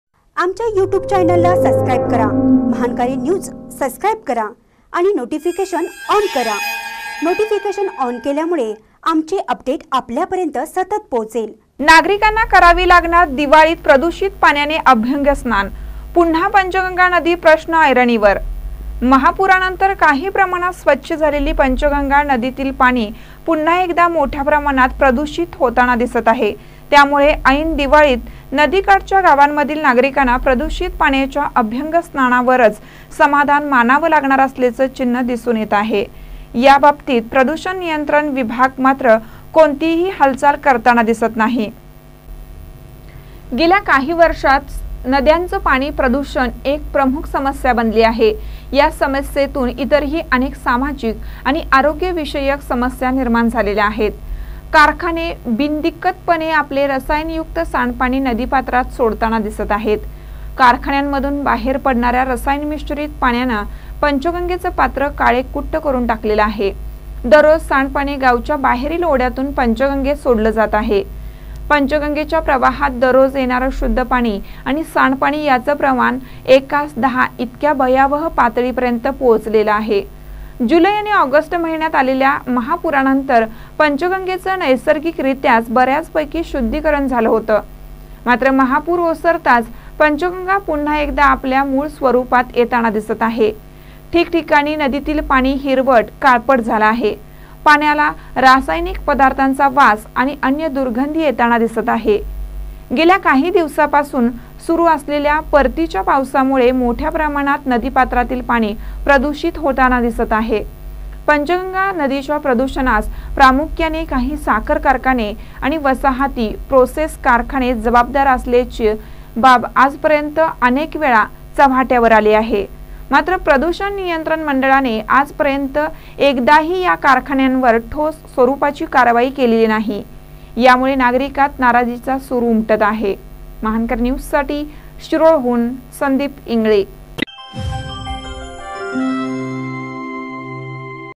नागरीकांना करावी लागणार दिवाळीत प्रदुषित पाण्याने अभ्यंगस्थान
नागरीकांना करावी लागणार दिवाळीत प्रदुषित पाण्याने अभ्यंगस्थान ; पुन्हा पंचगंगा नदी प्रश्‍न ऐरणीवर